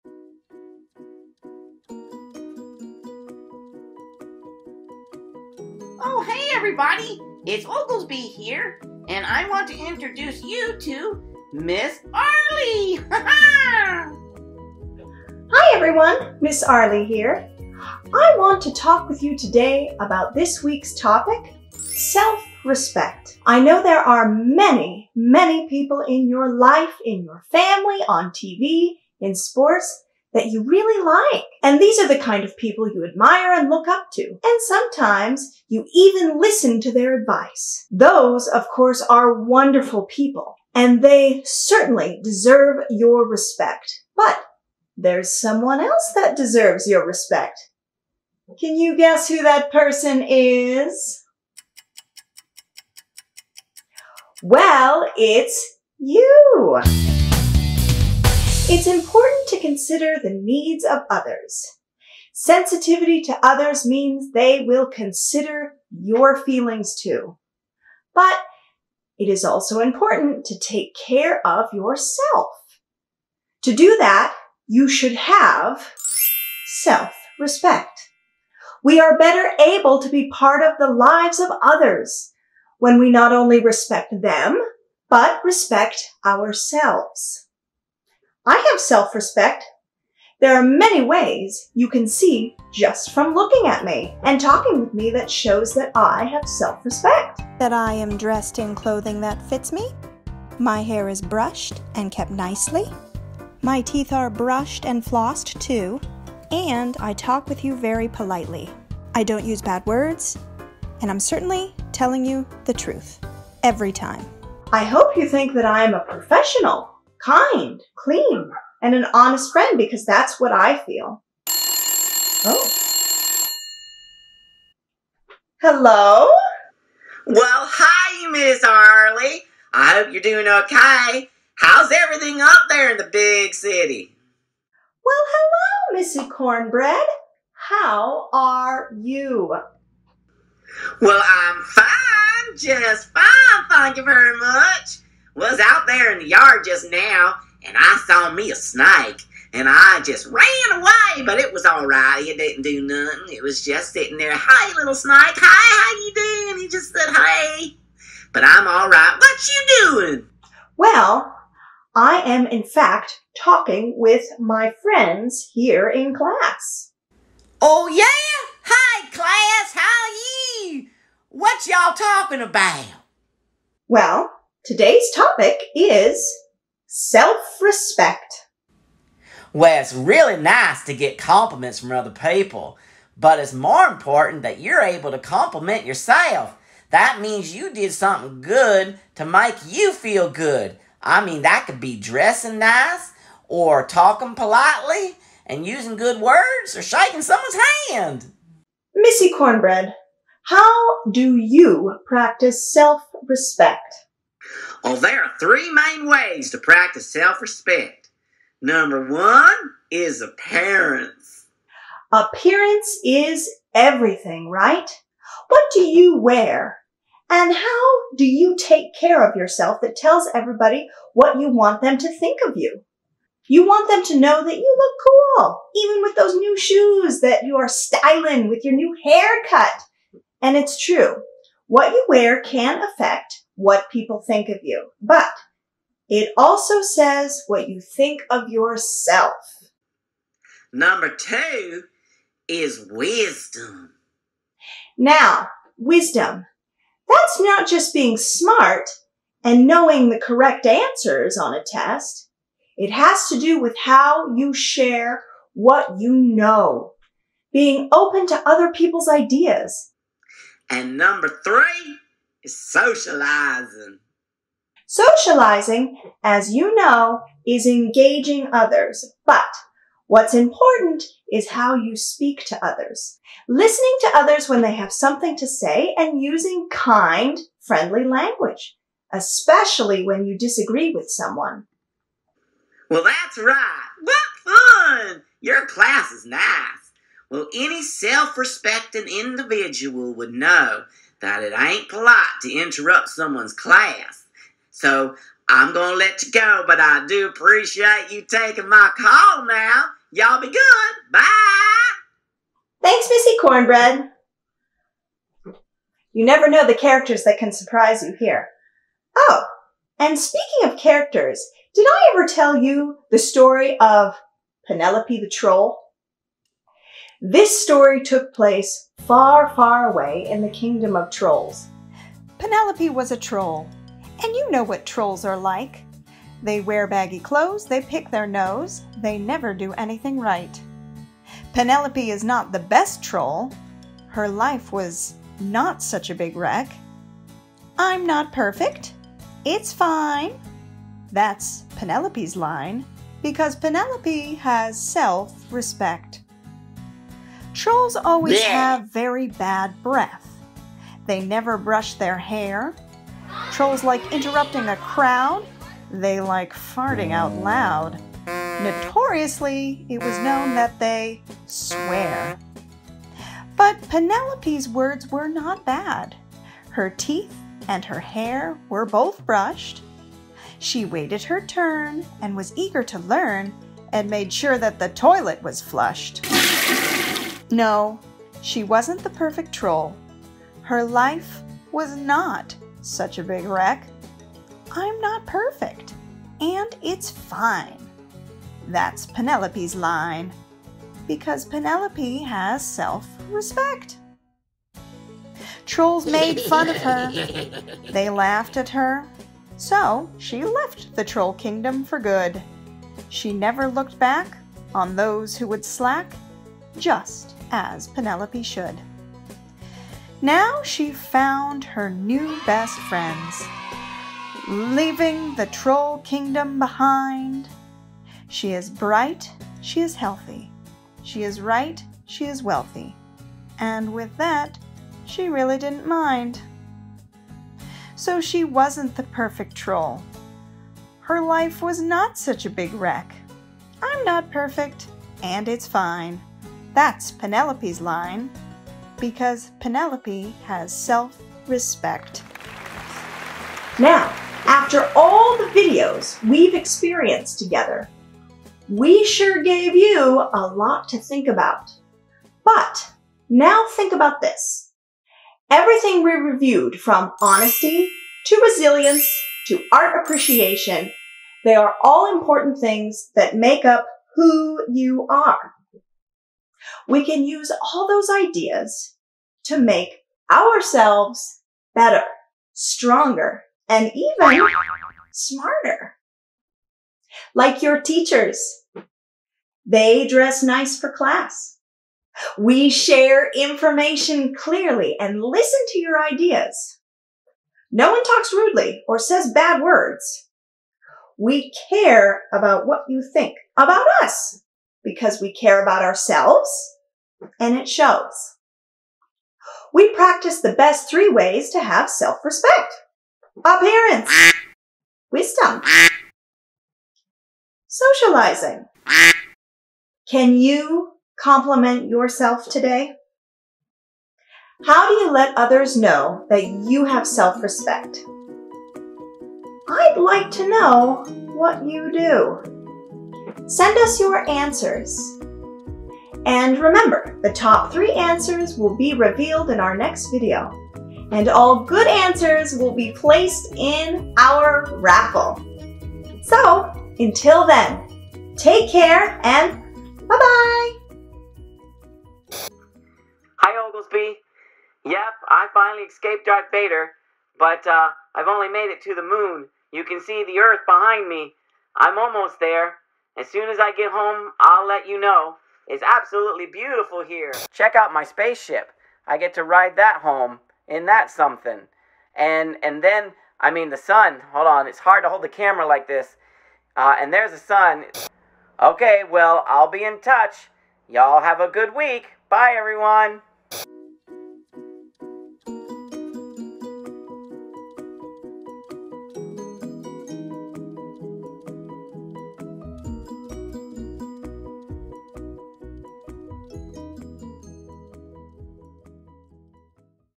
OWNERSHIP OF EDUCATION VOl: 9 Self-Respect
Ownership of Education Volume#9 Self-Respect.
Miss Arlee talks about ways to exercise self-respect from how you take care of yourself inside and out.